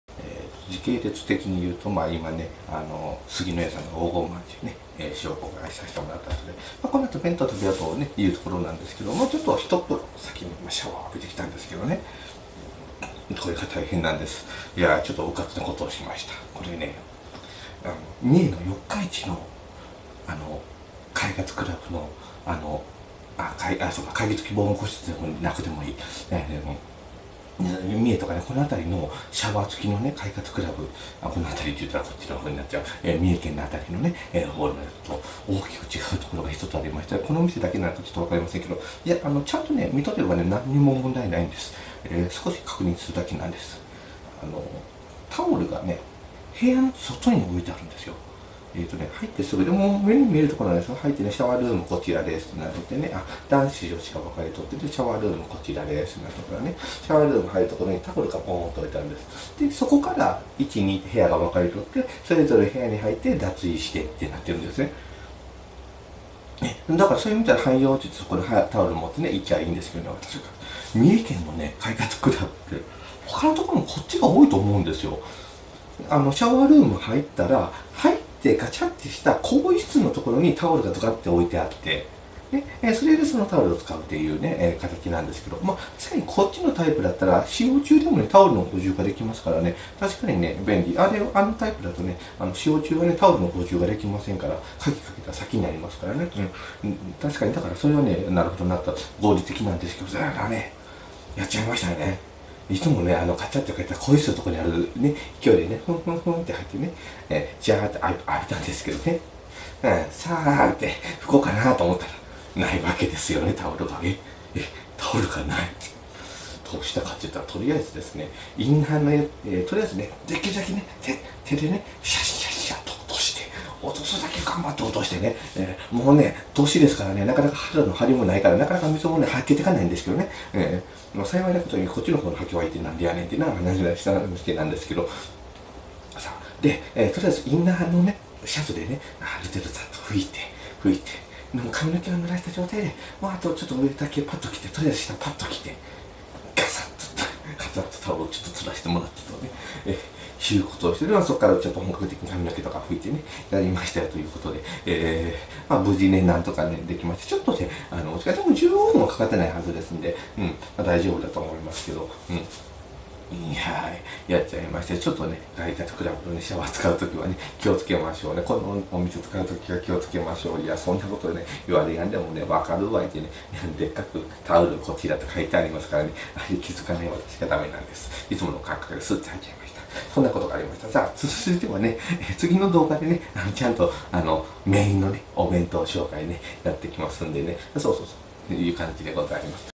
ST034’補1「快活CLUBのシャワールームに潜む、思わぬローカルルールの罠にかかった最底辺YouTuber」【静岡へ転勤する視聴者様の為に静岡市を徹底調査してみた】
シリーズのプレイリストをご用意しました

今回は閑話休題。
山田さんのケアレスミスにより、快活CLUBのシャワールームで起こった小さな悲劇のご紹介です。
ちゃんとちょっと、シャワールムに入る前に確認すれば何も問題なかったのに…

ちなみに快活CLUBのタオル置き場ってどっちが主流なんでしょうね？